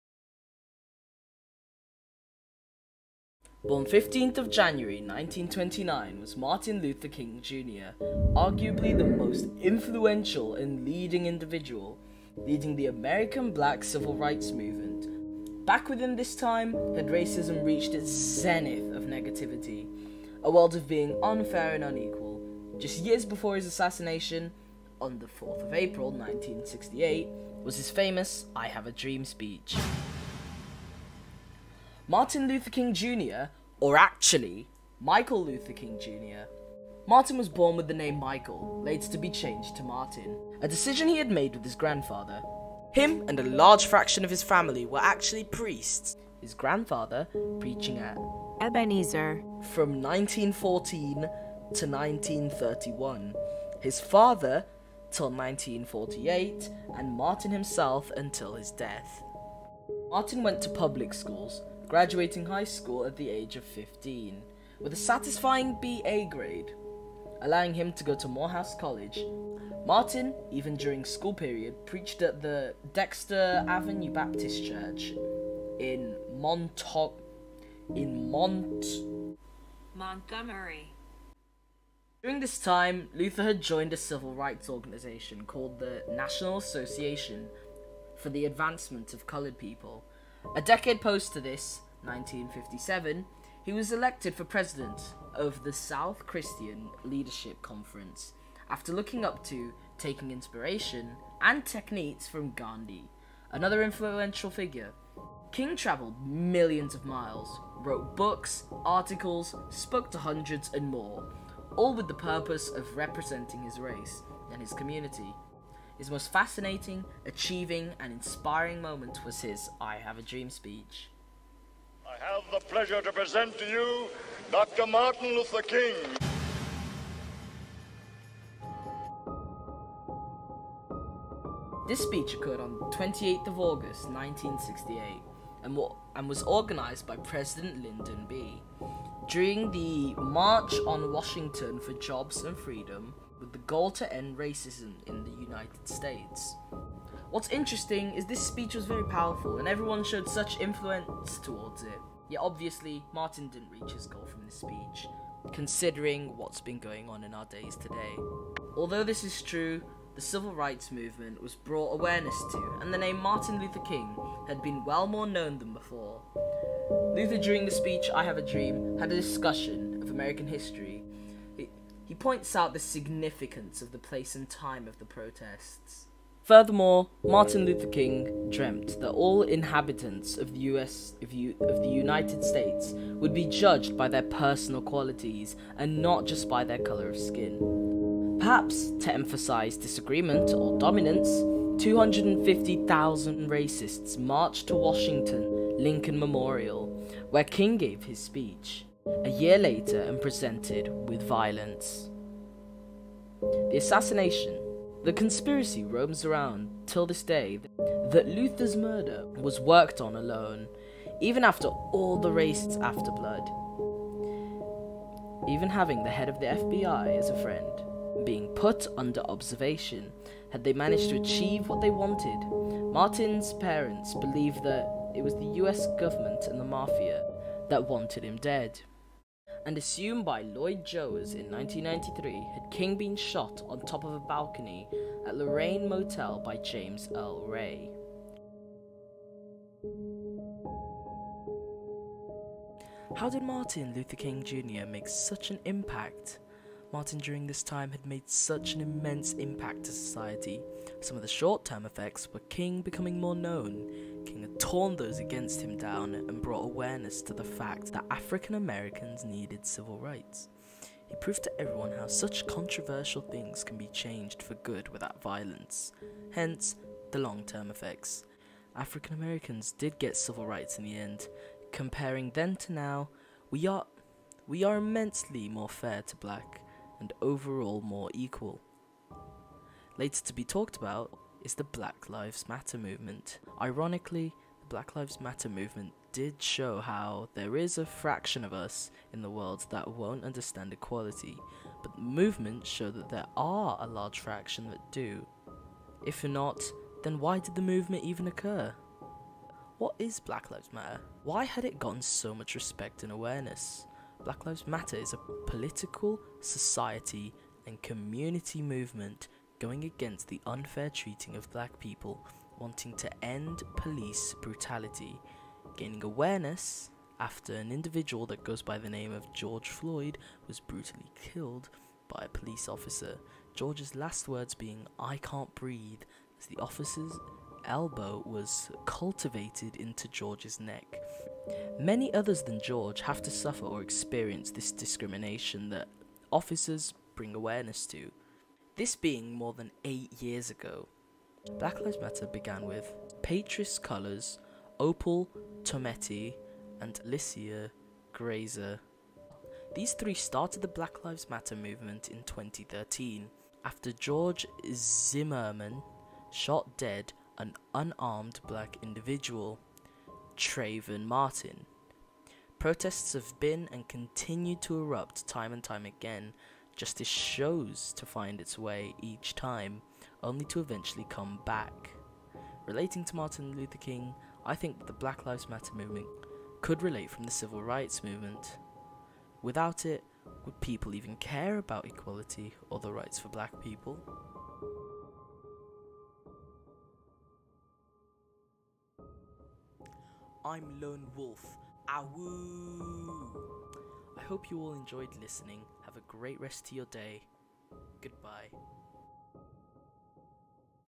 Martin Luther King Jr. | History Project
Script:
Born 15th January 1929, was Martin Luther King. Jr. Arguably the most influential and leading individual, leading the ‘American Black Civil Rights Movement’. Back within this time, had racism reached its zenith of negativity, a world of being unfair and unequal. Just years before his assassination (on 4th April 1968) was his famous ‘I Have A Dream’ speech.

Martin, went to public schools, graduating high school at the age of 15, with a satisfying B. A grade. Allowing him to go Morehouse College. Martin, even during school period, preached at the Dexter Avenue Baptist Church in Montgomery, Alabama. During this time Luther had joined a civil rights organization called the: ‘National Association for the Advancement of Colored People’. A decade post to this, 1957, he was elected president of the: ‘Southern Christian Leadership Conference’. After looking up to, taking inspiration, and techniques from Gandhi, another influential figure. King traveled millions of miles, wrote books, articles, spoke to hundreds and more. All with the purpose of representing his race and his community. His most fascinating, achievable and inspiring moment was his ‘I Have A Dream’ speech.

This speech occurred on the 28th August 1968 and was organized by President Lyndon B. During the “March on Washington for Jobs and Freedom” with the goal to end racism in the United States. What’s interesting is this speech was very powerful and everyone showed such influence towards it, yet obviously, Martin didn’t reach his goal from this speech. Although this is true, ‘The Civil Rights Movement’ was brought awareness too and the name ‘Martin Luther King’ had been well more known than before. Luther during the speech “I Have a Dream” has a discussion of American history. He points out the significance of the place and time of the protest. Furthermore Martin Luther King dreamt that all inhabitants of the United States would be judged by their personal qualities and not by the color of their skin. Perhaps to emphasize disagreement or dominance, 250,000 racists marched to Washington’s Lincoln Memorial (where King gave the speech) a year later and presented with violence.

The assassination. The conspiracy roams around today that Luthers murderer was working alone, even after all the racists after blood. Even after having the head of the FBI as a friend and being put under observation, had they managed to achieve what they wanted. Martins parents believe that it was the U.S government and mafia. And assumed by Loyd Jowers in 1993, had King been shot on top of a balcony at Lorraine Motel by James Earl Ray.

How did Martin Luther King Jr. make such an impact? Martin during this time had made such an immense impact to society. Some of the short term effects were King become more known, King had torn those against him down and brought awareness to the fact that African Americans needed Civil Rights. He proved to everyone how such controversial things can be changed for the good without violence. Hence, the long term effects. African Americans did get civil rights in the end. Comparing then to now, we are immensely more fair to the black and overall more equal. Later to be talked about, is the Black Lives Matter movement, how that was so important and reflective towards this time. Ironically, the Black Lives Matter Movement did show how there is a fraction of us in the world that won’t understand equality. But the movement shows that there are a large fraction that do, if not, then why did the movement even occur?

What is Black Lives Matter? Why had it gotten so much respect and awareness? Black Lives Matter is a political, society, and community movement, going against the unfair treating of black people; wanting to end police brutality. Gaining awareness after an individual that goes by the name of: George Floyd was brutally killed by a police officer. George’s last words being “I can’t breathe” as  the officer’s elbow was cultivated into George’s neck. Many other than George having to suffer or experience the discrimination of these officers had been brought awareness too, this being over almost 8 years. Black Lives Matter began with Patrisse Cullors and Opal Tometi and Alicia Garza, stating that they have changed politics. These tree started the Black Lives Matter movement in 2013 after George Zimmerman shot dead unarmed black individual Trayvon Martin. Protests have been and continue to erupt time and time again. Justice shows to find its way each time, only to eventually come back. Relating to Martin Luther King, I think that the Black Lives Matter movement could relate from The Civil Rights Movement, maybe without it, would people not care about equality and the rights for black people.